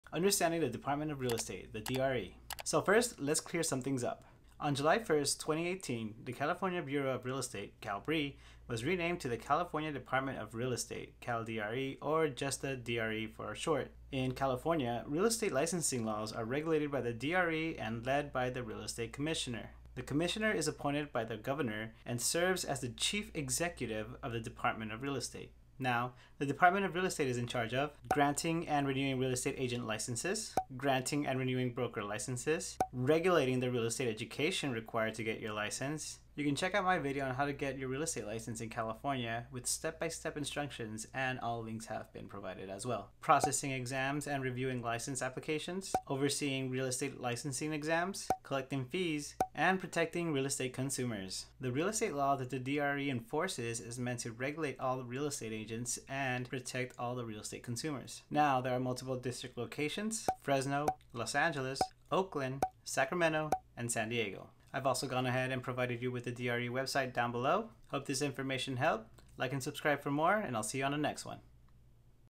Understanding the Department of Real Estate "DRE"
A quick summary of  the Department of Real Estate "DRE" and their responsibilities.

🔹ALL OF THE YOUTUBE EQUIPMENT I USE🔹